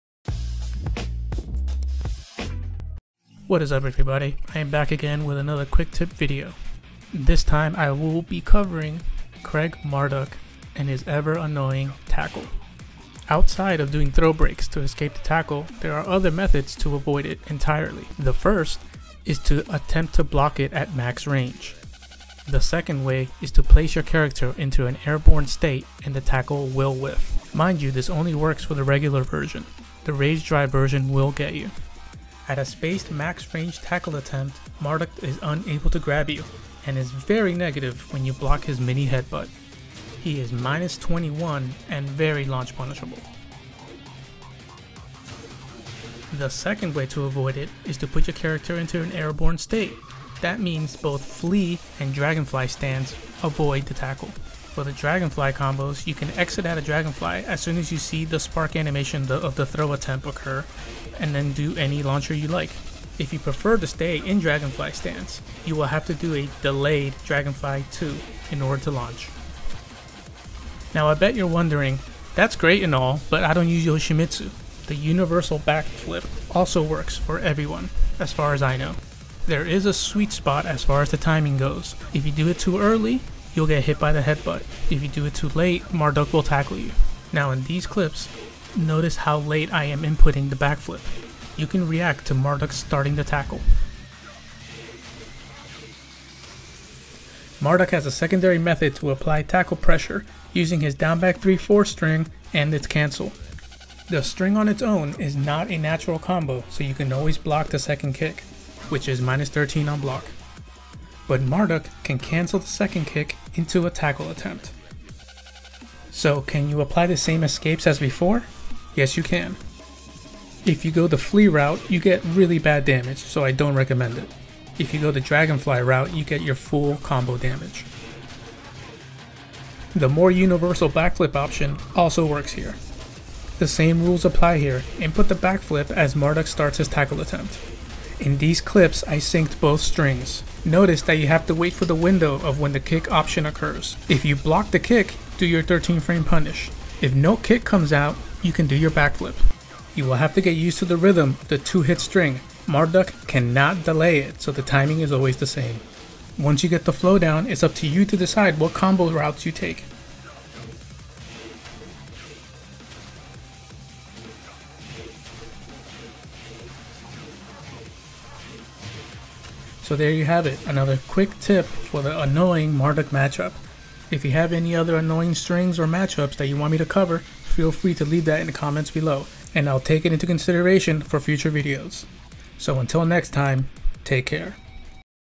Yoshimitsu Tips from the vault. #3 - Anti-Marduk VTS Tackle
This weeks Tips from the Vault is how to handle Craig Marduk's VTS Tackle.